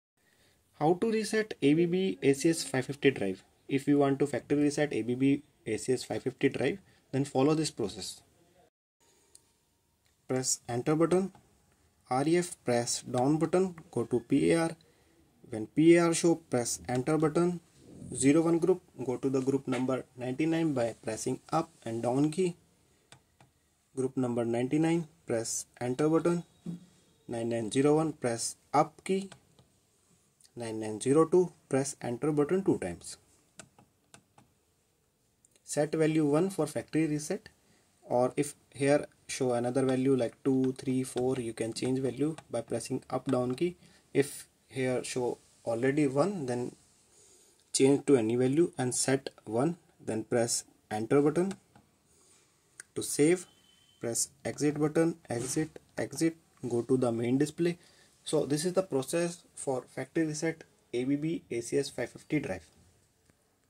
ABB ACS550 drive factory reset Parameter in english
Restore your ABB ACS550 drive to its original factory settings. Learn how to perform a factory reset to troubleshoot issues, reconfigure the drive, or start from scratch. This guide provides clear instructions on accessing and executing the factory reset parameter.